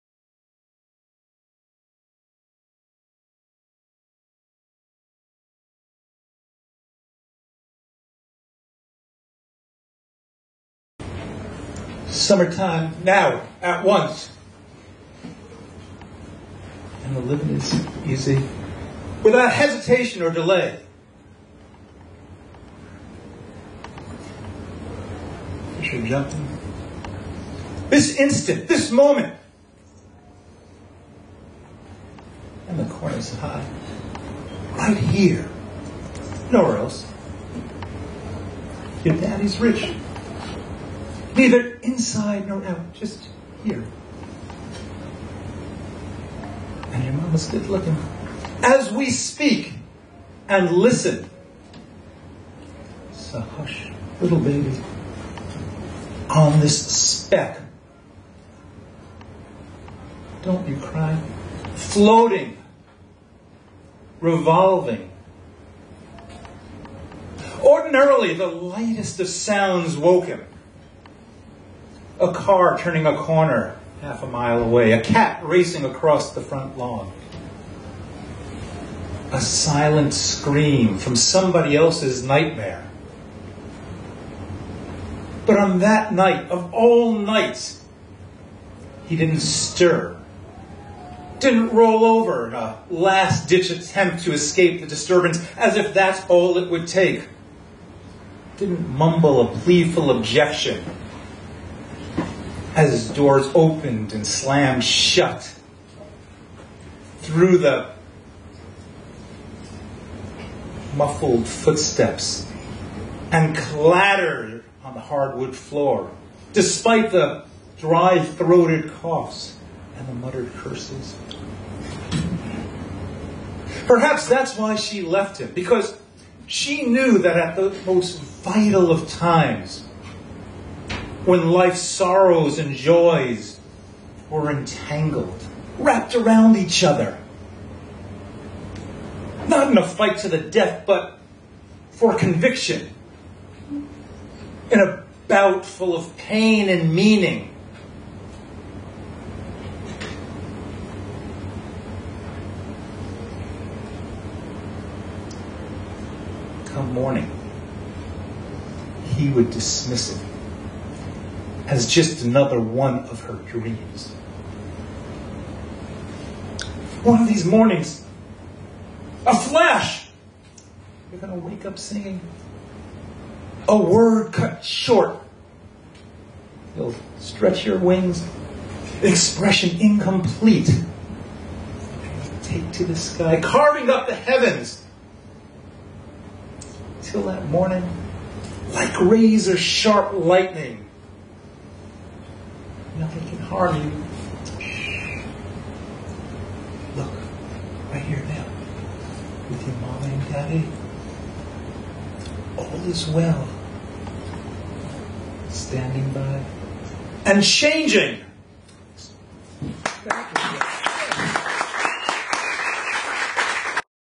"Open Mic Gypsy" Zev Torres for Rimes of The Ancient Mariner October 3, 2013
"Open Mic Gypsy" Zev Torres for Rimes of The Ancient Mariner October 3, 2013 @ Identity Bar & Lounge.